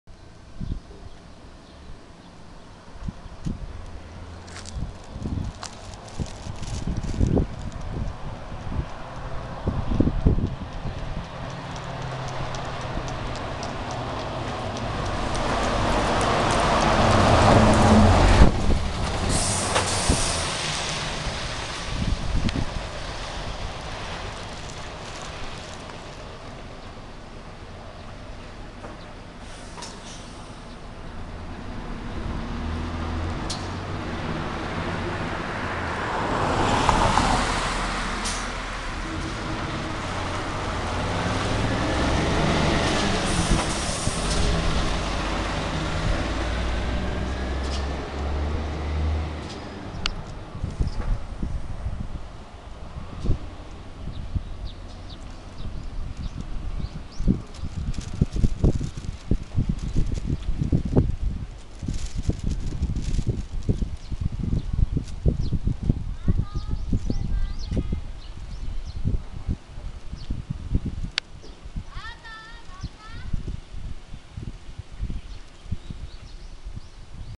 防長交通路線車(津和野バスセンター行き)・津和野駅前バス停到着後すぐ発車
2021年5月9日撮影。
防長交通の東萩駅発津和野バスセンター行きの路線車ですが、乗降客がいなかったため到着後戸を開けることなく直ぐに発車してしまいました。
車種は日野自動車製のブルーリボンシティ、近鉄バスからの中古車だったと思います。